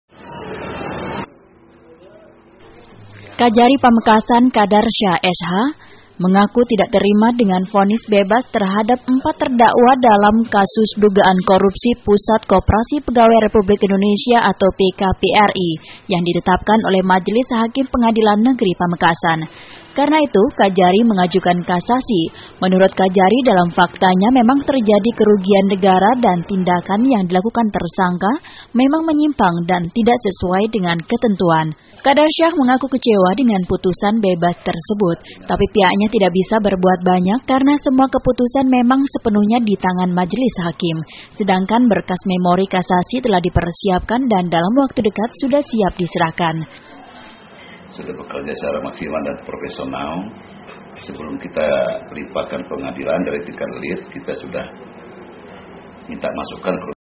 KASASI DAN SIDANG PUTUSAN PKPRI
kejaksaan negeri pamekasan akhirnya memastikan akan melakukan kasasi ke mahkamah agung (ma). kasasi dilakukan  atas vonis bebas empat terdakwa kasus dugaan korupsi pusat koperasi pegawai republik indonesia (pkpri) yang ditetapkan oleh majelis hakim pengadilan negeri (pn) pamekasan. Berita madura, berita sumenep, berita pamekasan, berita sampang, berita bangkalan, berita terbaru, howto, most, how to do